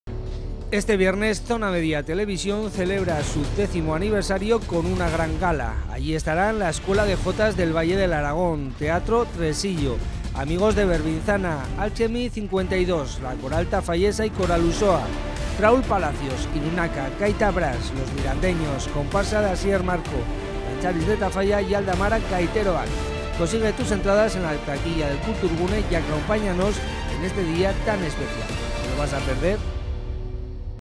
Consigue tus entradas · Gala 10º Aniversario Zona Media Tv
🔥La gala de nuestro 10º aniversario está a la vuelta de la esquina🔥
¿Todavía no tienes tu entrada?
A que esperas consíguela en la taquilla del Kulturgune y acompáñanos en este día tan especial

Te esperamos este viernes 18 a las 19:00 en el Kulturgune